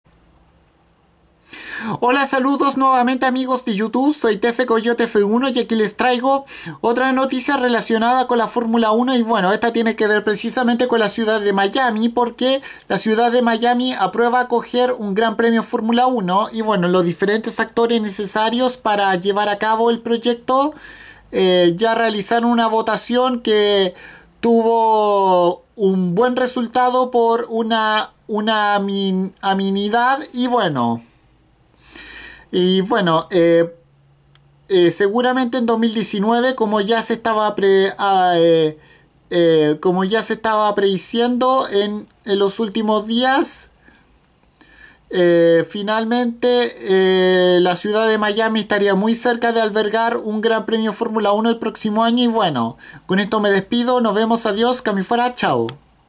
La ciudad de Miami aprueba el futuro Gran Premio de Formula 1
los diferentes actores necesarios para llevar a cabo el nuevo proyecto hicieron una votación que consiguió buenos resultados